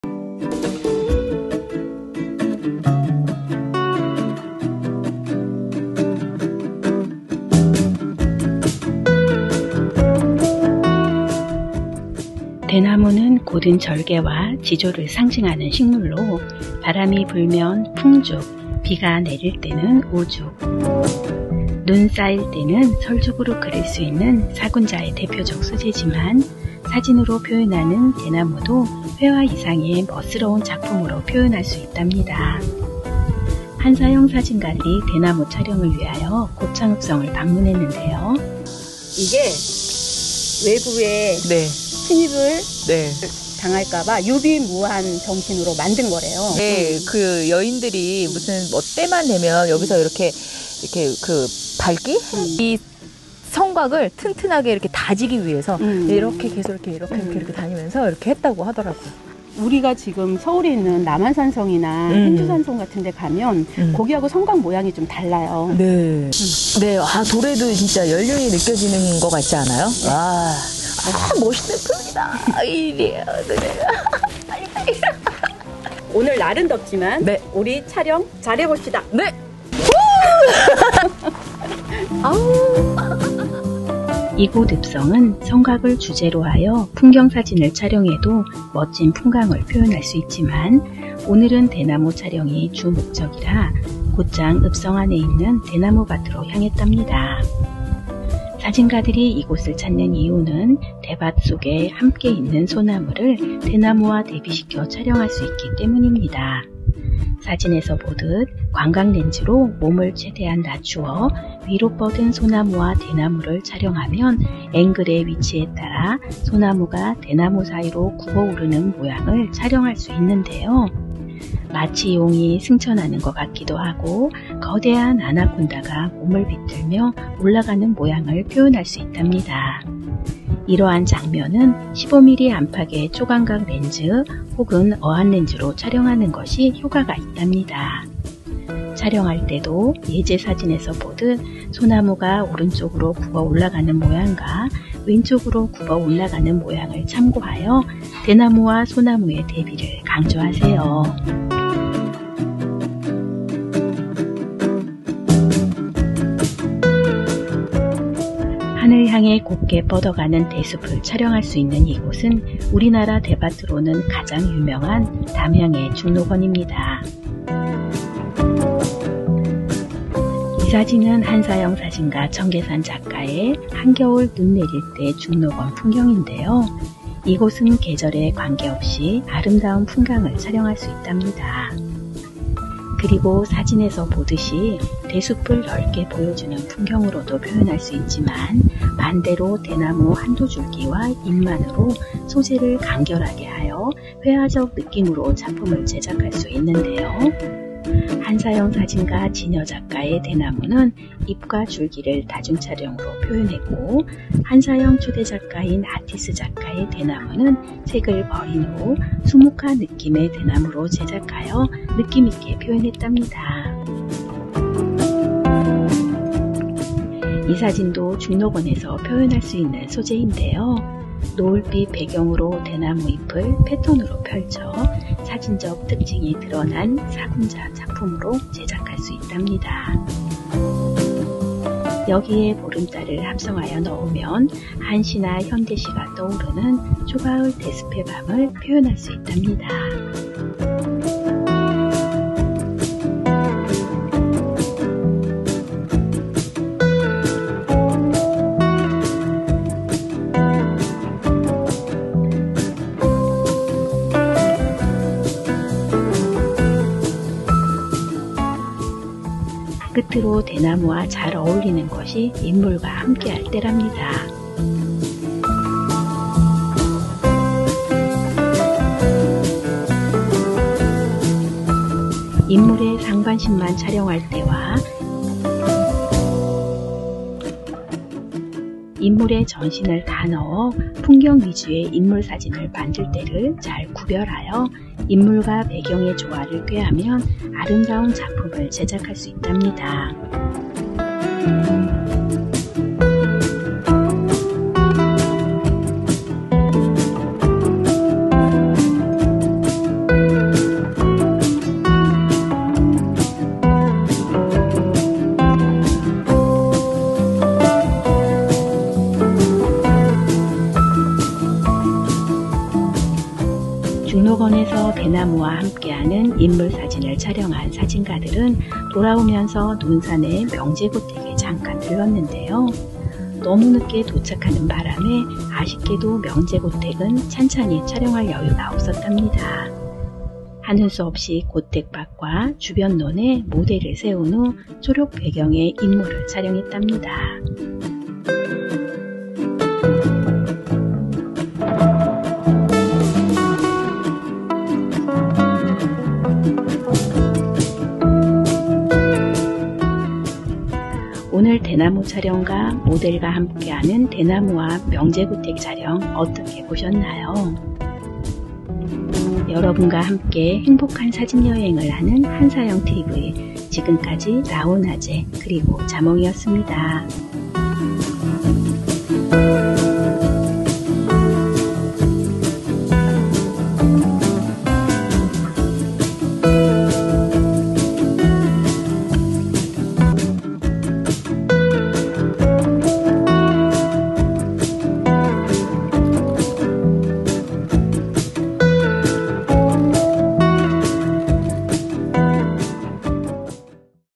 국내여행_담양 고창읍성 죽녹원에서 대나무 촬영하기_사진강의_한사영TV
담양 고창읍성 죽녹원에서
기가막힌 성벽과 대나무를 보고 왔습니다!
오늘의 사진 감상해볼까요?